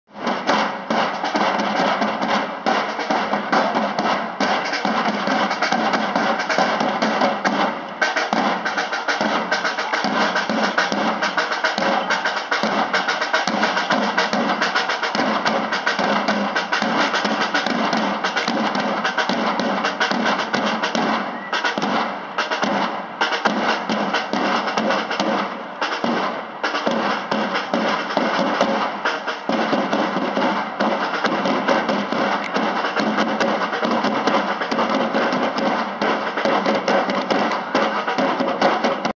20150106 Milano - Ingresso Sbandieratori
20150106 Milano - Ingresso Sbandieratori . in occasione del corteo dei Re Magi, presso la Basilica di Sant'Eustorgio.